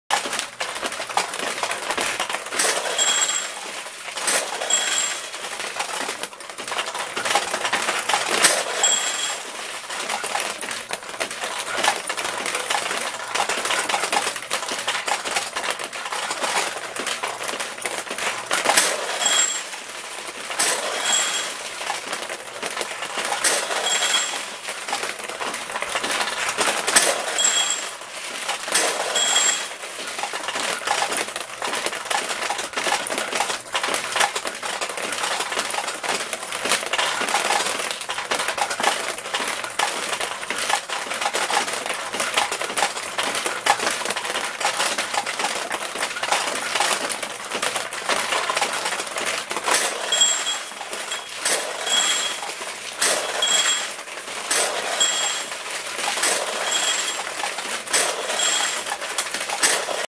1975 Nishijin Pink Crab
This is a vintage 1975 Nishijin model 4200 “AKA model B" Pachinko Machine. This machine is known as “Pink Crab” as the main attraction has a pink crab on it. The background is the "Pink NVEC" which provides a very colorful playfield. It is a semi-automatic AKA non-recirculating pachinko machine